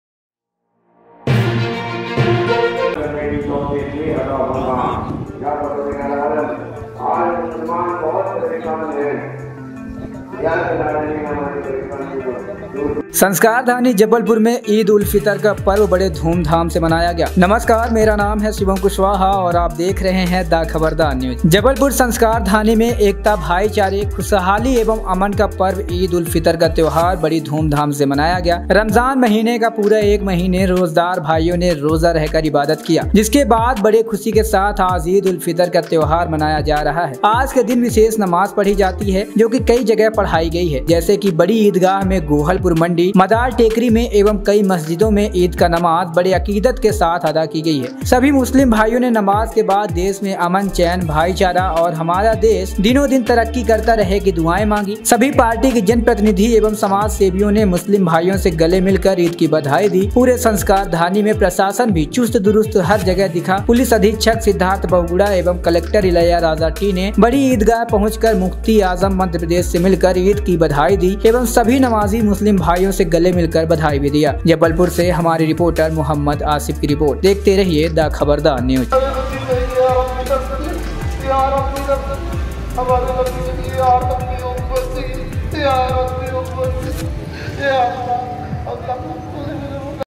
संस्कारधानी जबलपुर में ईद उल फितर का पर्व बड़े धूमधाम से मनाया गया
एंकर
जबलपुर संस्कारधानी में एकता भाईचारे खुशहाली एवं अमन का पार्व ईद उल फितर का त्यौहार बड़ी धूमधाम से मनाया गया रमजान महीने का पूरा 1 महीने रोजदार भाइयों ने रोजा रहकर इबादत किया जिसके बाद बड़े खुशी के साथ आज ईद उल फितर का त्यौहार मनाया जा रहा है
आज के दिन विशेष नमाज पढ़ी जाती है जो कि कई जगह पढ़ाई गई है जैसे कि
बड़ी ईदगाह में गोहलपुर मंडी मदार टेकरी में  एवं कई मस्जिदों में ईद का नमाज बड़े अकीदत के साथ अदा की गई सभी मुस्लिम भाइयों ने नमाज के बाद देश में अमन चैन भाईचारा और हमारा देश दिनों दिन तरक्की करता रहे की दुआएं मांगी सभी पार्टी के जनप्रतिनिधि एवं समाजसेवियों ने मुस्लिम भाइयों से गले मिलकर ईद की बधाई दी
पूरे संस्कारधानी में प्रशासन भी चुस्त-दुरुस्त हर जगह दिखा पुलिस अधीक्षक सिद्धार्थ बहुगुणा एवं कलेक्टर इलैयाराजा टी ने बड़ी ईदगाह पहुंचकर मुफ्ती आजम मध्यप्रदेश से मिलकर ईद की बधाई दी एवं सभी नामाजी मुस्लिम भाइयों से गले मिलकर बधाई भी दिया

*जबलपुर से मोहम्मद आसिफ की रिपोर्ट*